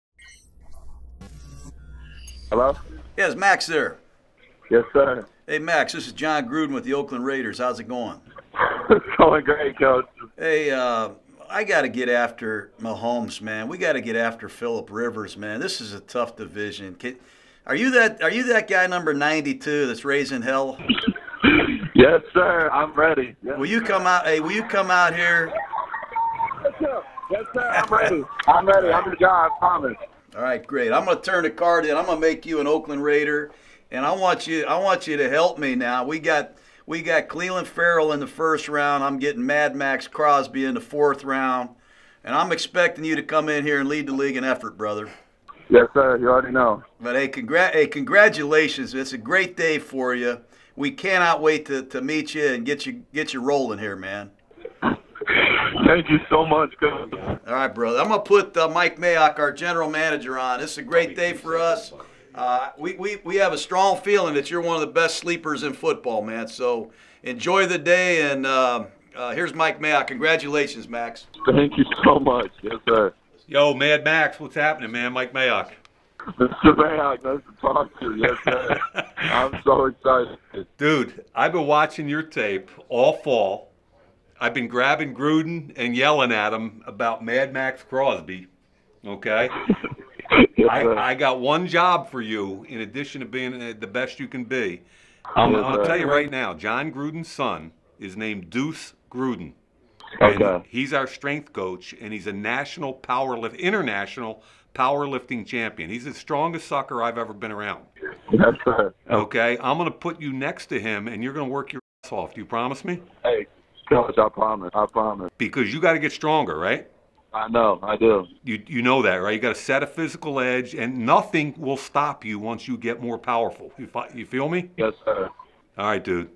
The Call: DE Maxx Crosby
Listen in as General Manager Mike Mayock and Head Coach Jon Gruden call defensive end Maxx Crosby to let him know he is being drafted by the Raiders.